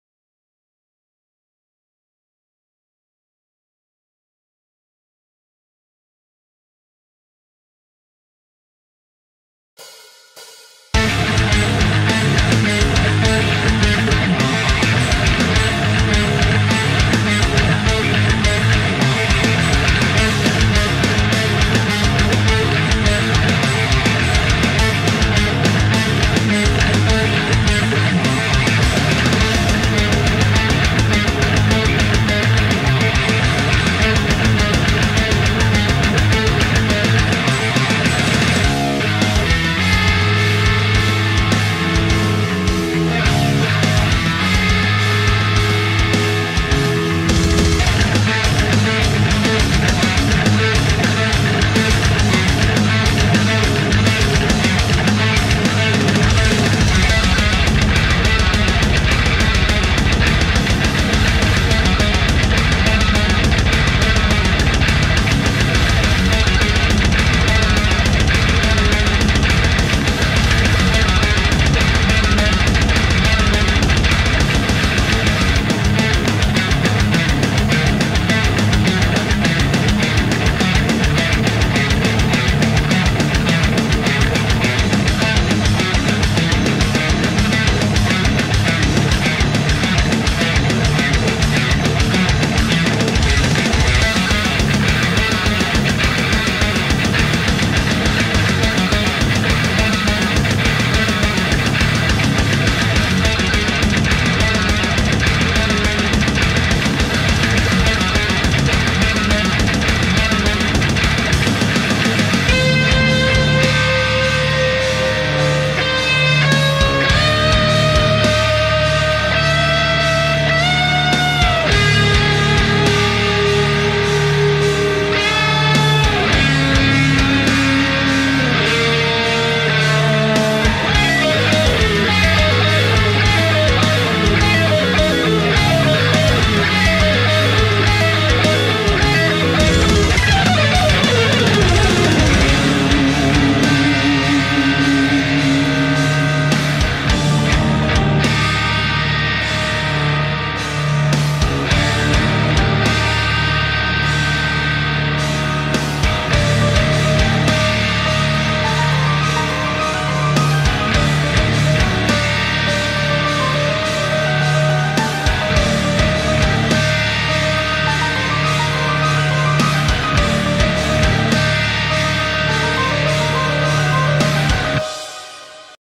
Orange Dark Terror Head - Hi Gain Metal Test - Fender Telecaster
My Short demo on Orange Dark Terror Amp

Gear Used.:
Fender telecaster
Laney Irt cab 2-12
Zoom g5
Focusrite 18i8
Reaper
Nikon D5500